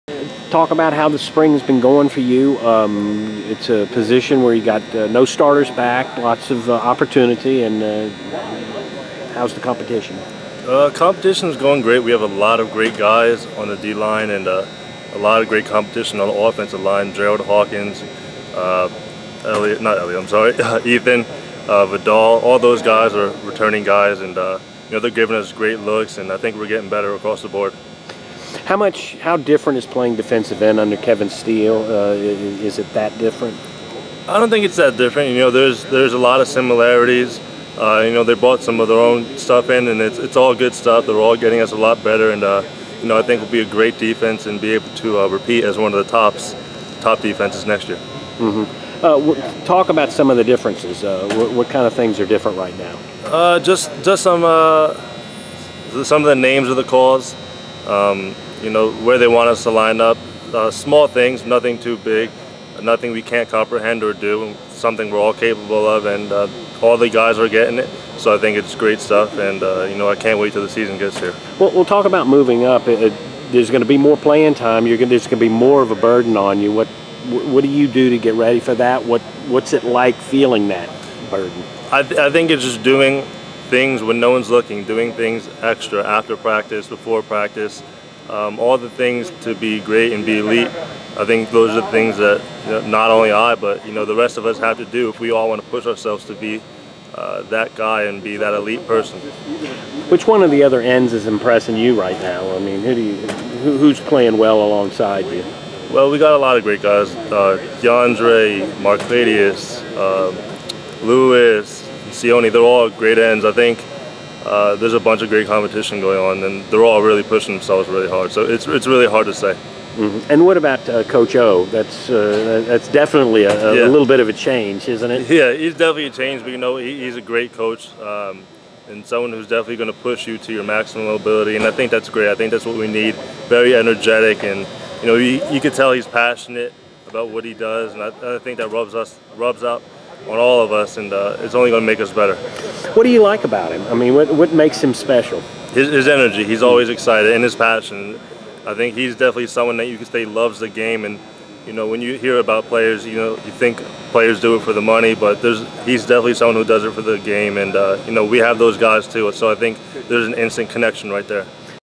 LSU DE Tashawn Bower says battle for starting job is intense | Video
LSU defensive end Tashawn Bower talks to the media.